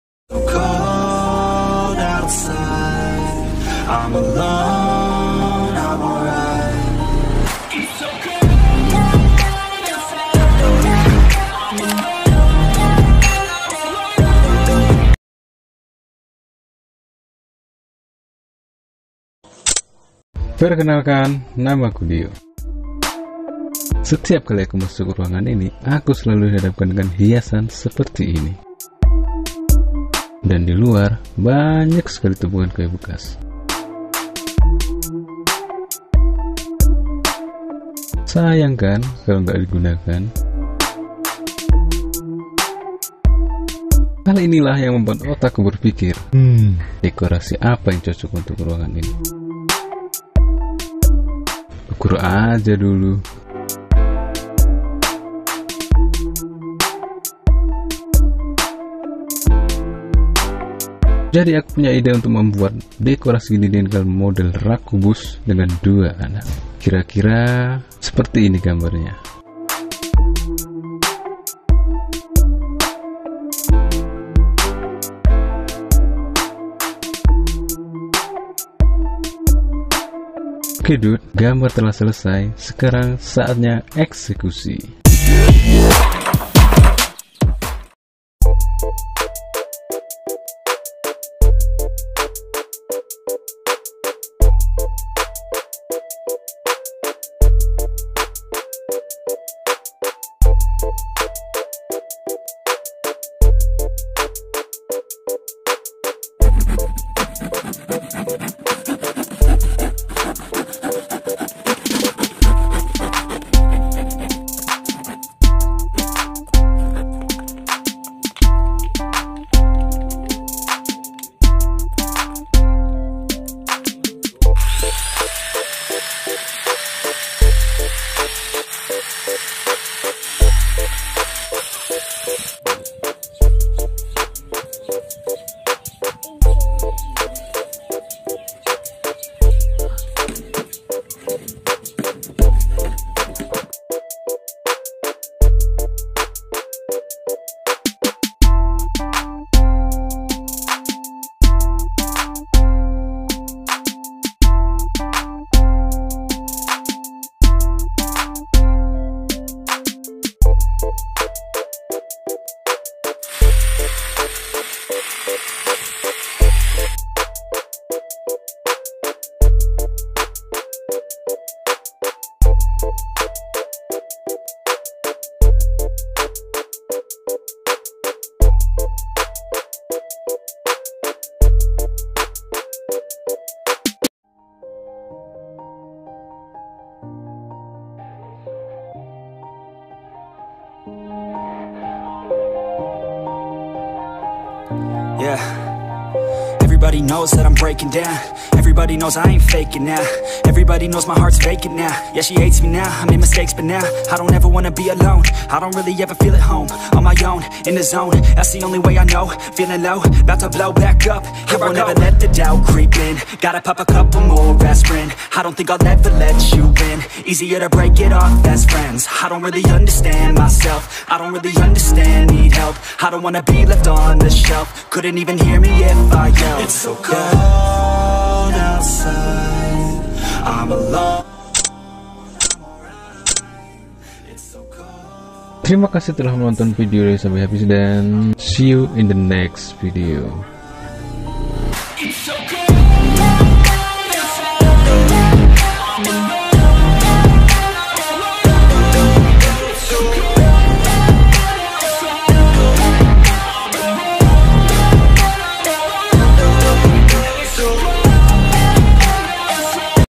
interior ruang tamu dari limbah triplek bekas
Meskipun dari kayu bekas, akan tetapi jika kita bisa mengolahnya hasilnya akan sangat memuaskan, tidak perlu keluar biaya banyak karena anda sendiri bisa membuatnya, simpel, mudah, praktis
 Ikuti terus chanel ini karena masih banyak hal menarik lain yang akan kita bahas